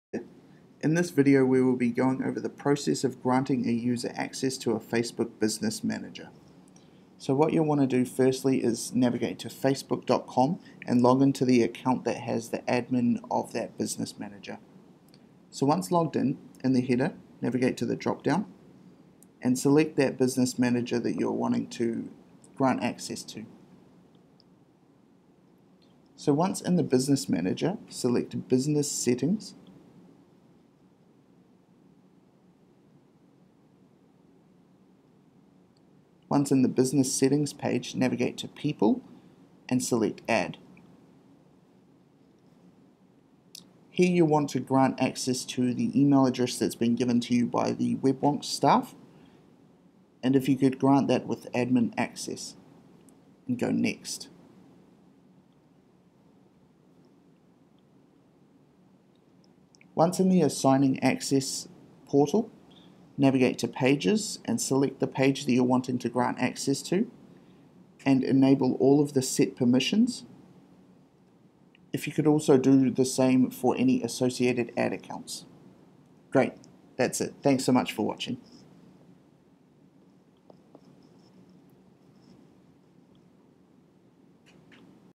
How To Add a User to Facebook Business Manager
In this short video, we show you how to add a user to a Facebook Business Manager Account.